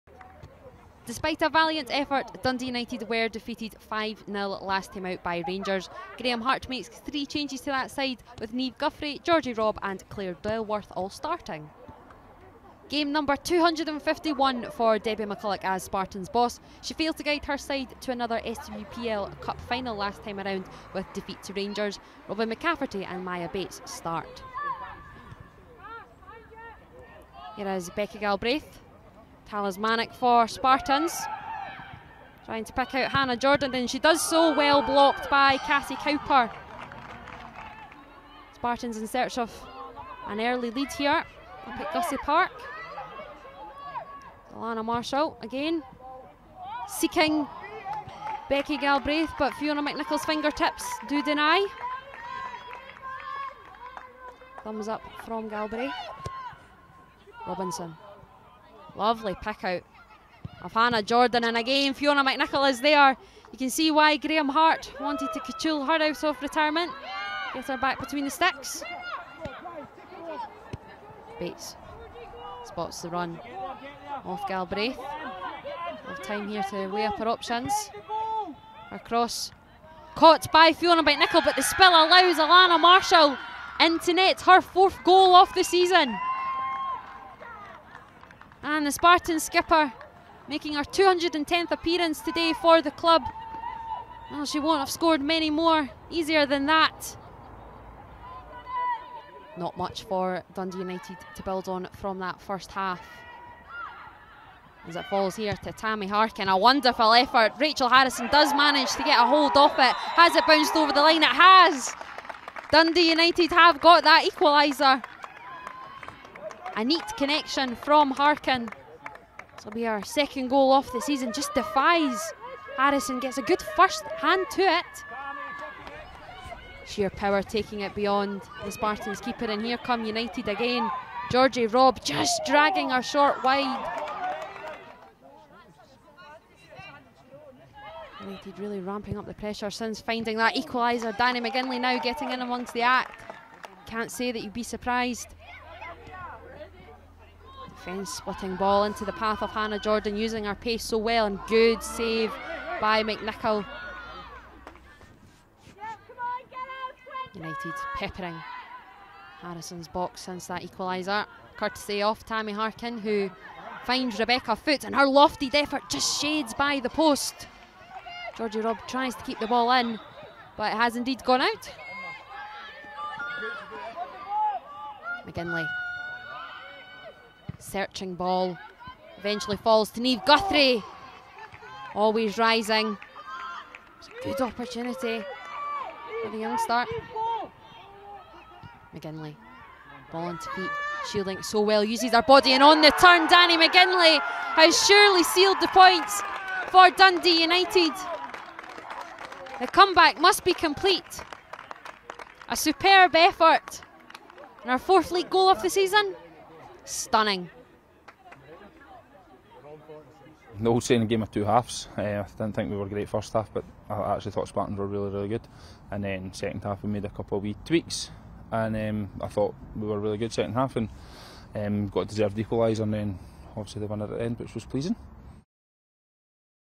Dundee United 2-1 Spartans | United's last gasp winner moves them above opponents | SWPL
Dundee United are now six points above 11th place with an important victory over established Spartans. The result moves Spartans down to 9th in the table but they'll be strong favourites for three points when they face Glasgow Women this coming Sunday.

The top leagues are called the Scottish Women's Premier League (SWPL) and SWPL 2.